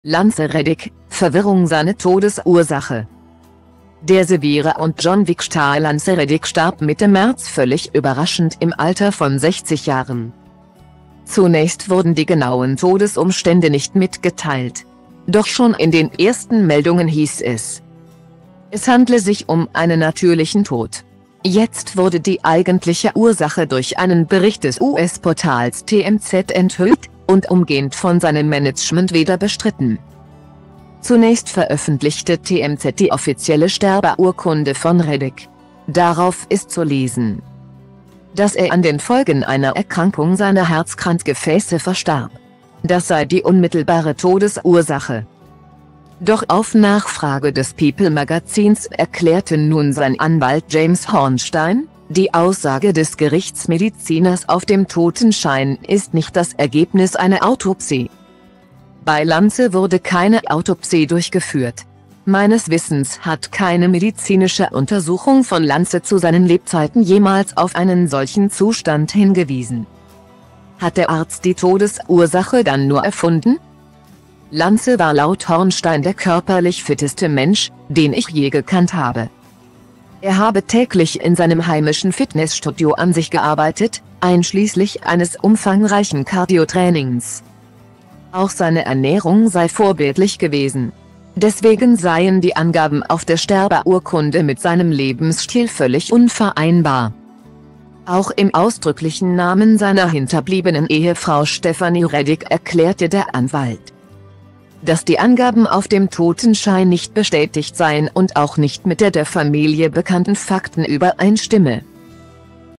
Lance Reddick: Verwirrung seine Todesursache
Der "The Wire" und "John Wick"-Star Lance Reddick starb Mitte März völlig überraschend im Alter von 60 Jahren.Zunächst wurden die genauen Todesumstände nicht mitgeteilt. Doch schon in den ersten Meldungen hieß es,es handele sich um einen natürlichen Tod. Jetzt wurde die eigentliche Ursache durch einen Bericht des US-Portals "TMZ" enthüllt - und umgehend von seinem Management wieder bestritten.